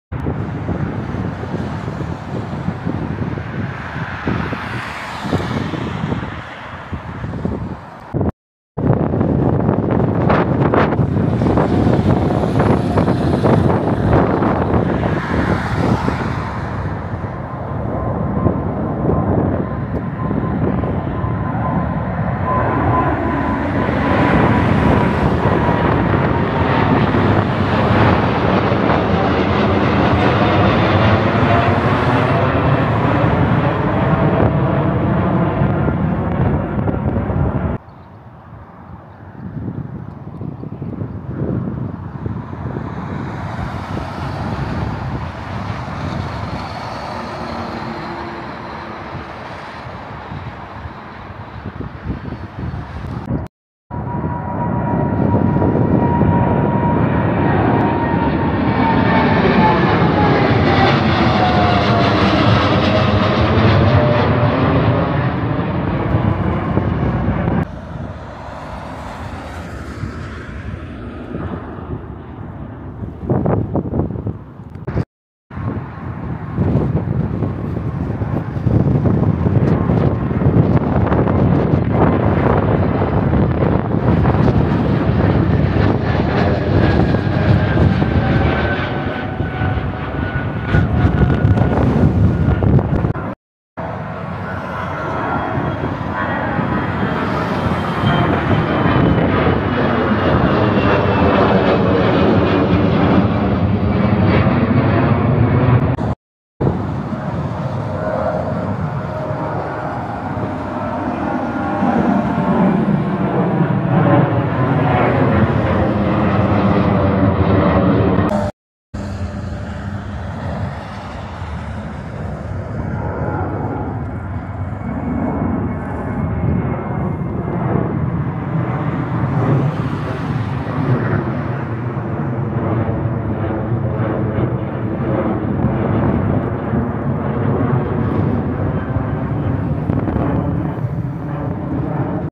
Plane spotting point near Toronto YYZ Pearson International Airport | 4K | Canada
Plane spotting point at the Dixie road in Mississauga right outside the Toronto YYZ Pearson International Airport, one of the busiest airports. Usually people come there and wait in their cars to get a good close view of flights taking off from the airport. I could spot around 8 planes taking off in 15 minutes. Ideal place to spend a quick 15 to 30 minutes on a lazy afternoon.